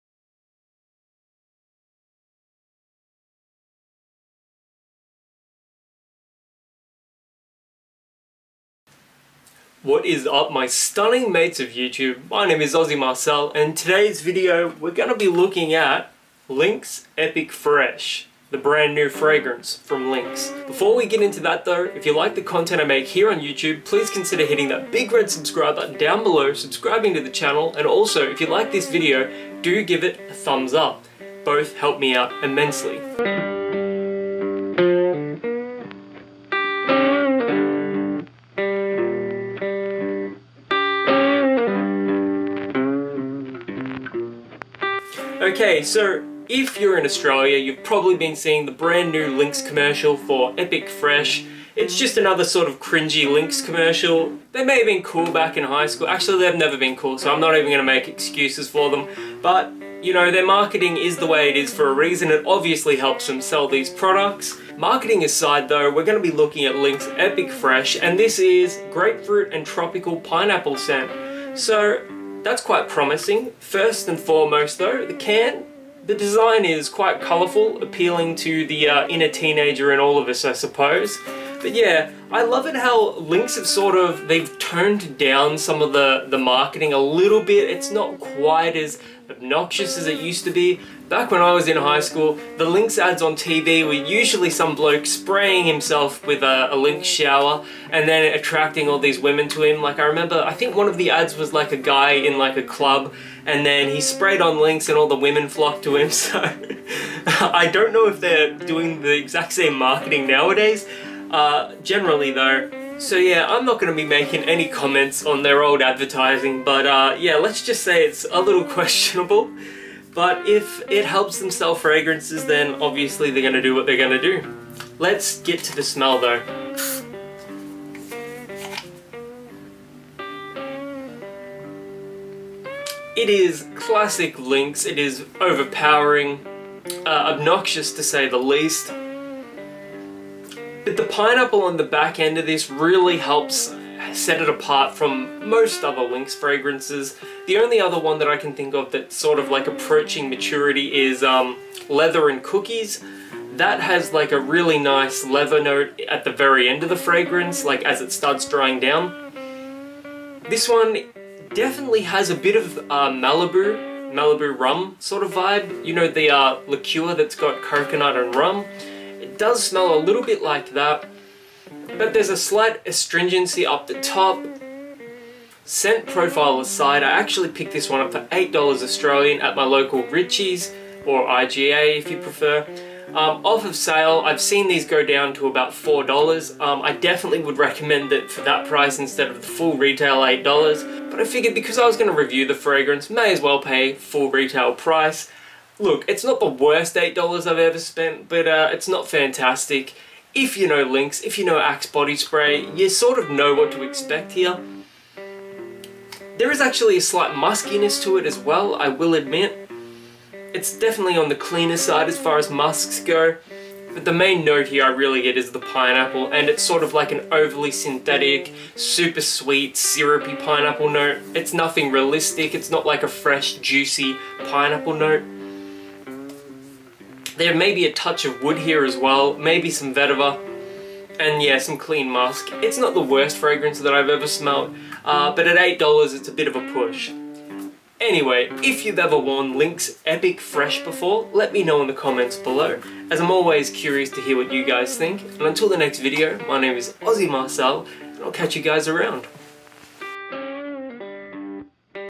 Lynx Epic Fresh (Axe Epic Fresh) Men's Body Spray First Impressions
In today's video I'll be giving my first impressions on the new Lynx / Axe body spray, Epic Fresh. Enjoy.